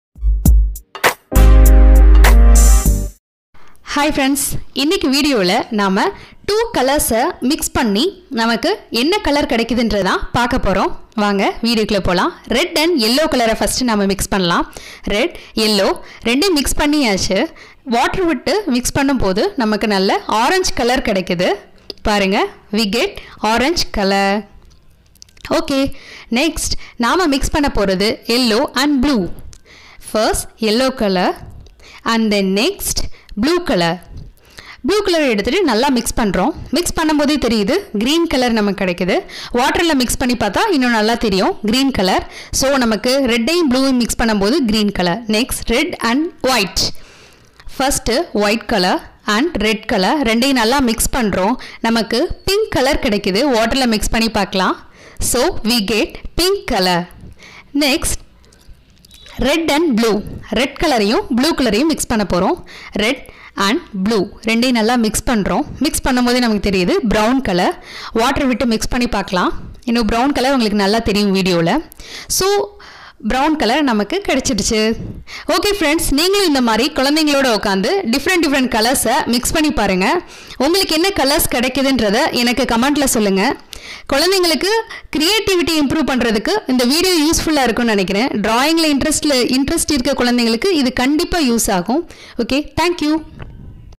Summer time activities/play with colours
Summer time activities/play with colours/Red+yellow=orange/Red+white=pink/Red+blue=brown/yellow+blue=green